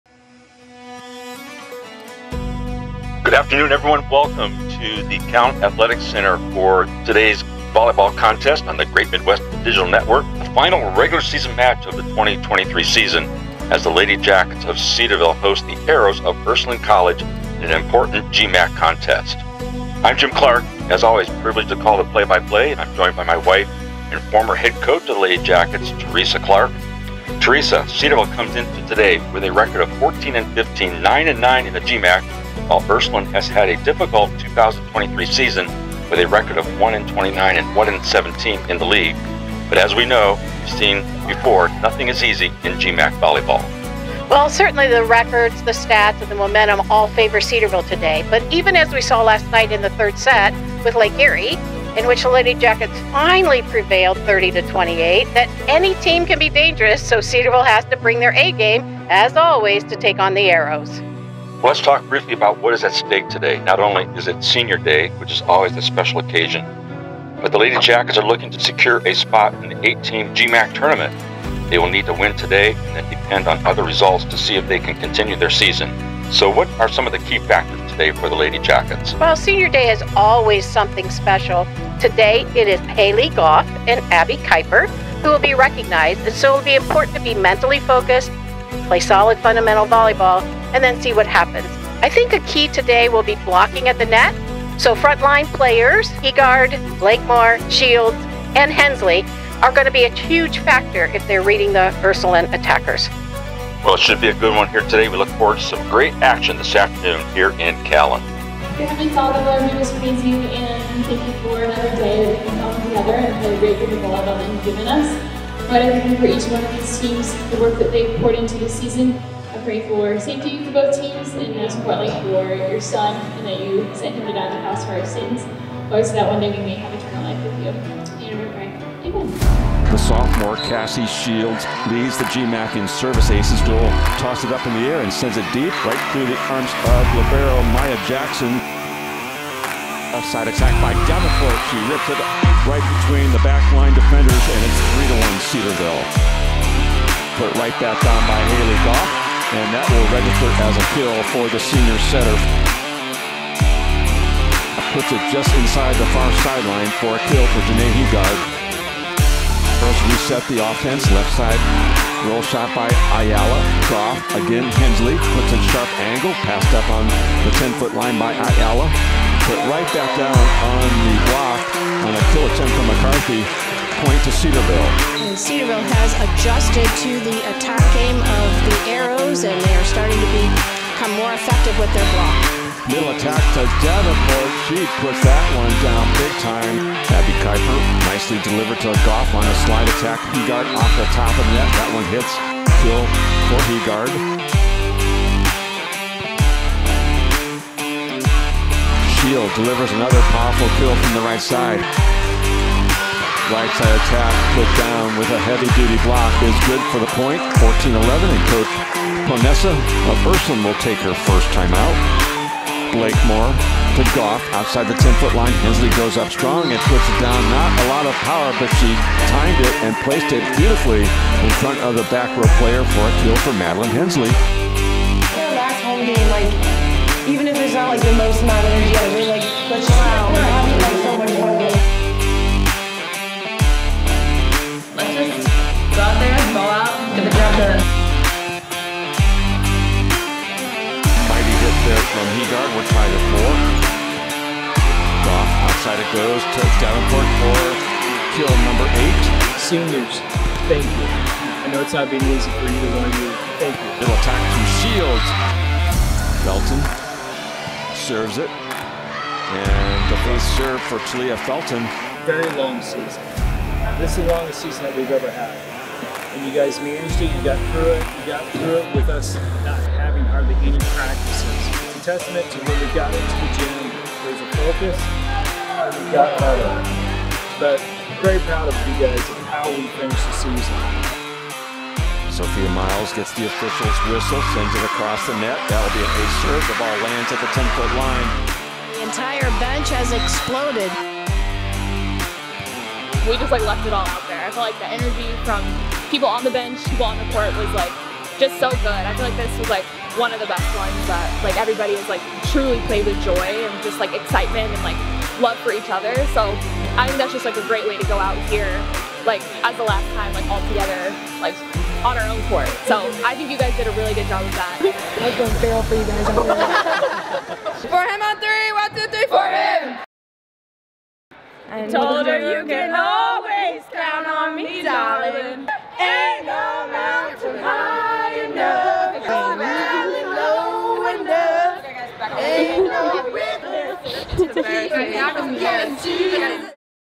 Cedarville Volleyball: the short film
Cedarville Volleyball against Ursuline senior night